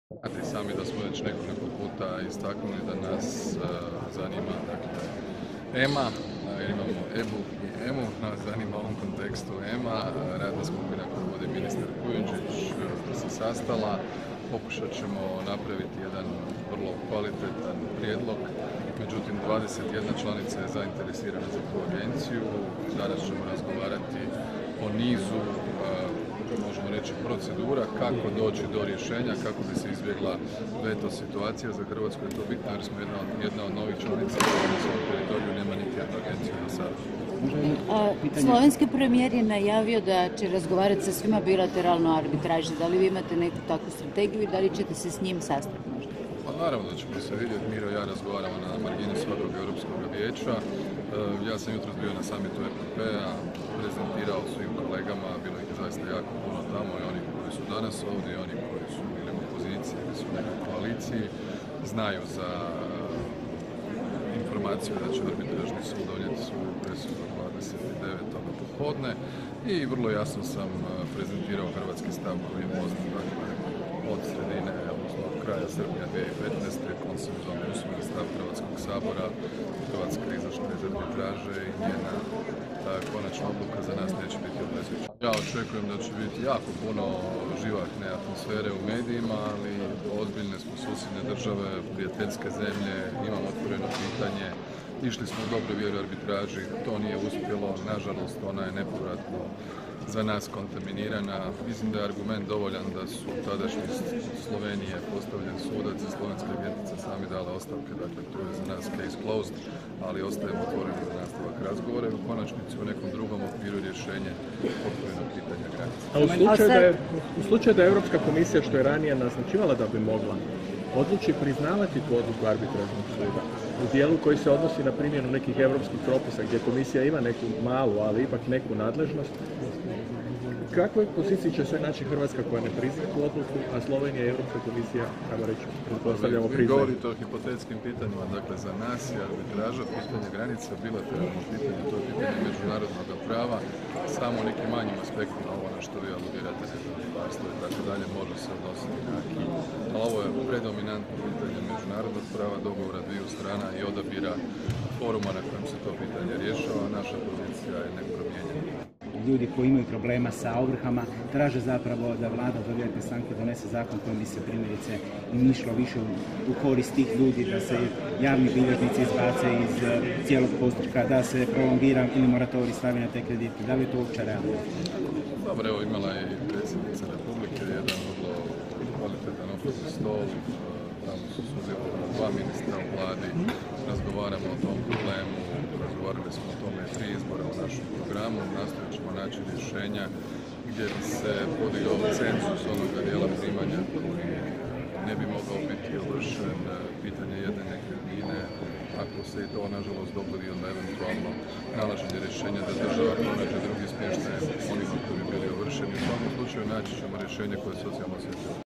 Plenković: Hrvatska će napraviti kvalitetan prijedlog za sjedište Europske agencije za lijekove
Bruxelles, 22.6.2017 - Hrvatski premijer Andrej Plenković izjavio je u četvrtak da će Hrvatska pripremiti kvalitetan prijedlog za udomljavanje Europske agencije za lijekove (EMA), koja se zbog Brexita mora preseliti iz Londona i za koju vlada veliki interes među zemljama članicama. "Mi smo više puta istaknuli da nas zanima EMA i pokušat ćemo napraviti jedan kvalitetan prijedlog. Međutim, 21 zemlja članica je zainteresirana za to", rekao je Plenković prije početka summita čelnika EU-a, na kojem će jedna od tema biti pitanje udomljavanja dviju europskih agencija, koje sada imaju sjedište u Londonu - Europske agencije za lijekove i Europskog nadzornog tijela za bankarstvo (EBA).
video HINA/ Slavko VUKADIN/ ua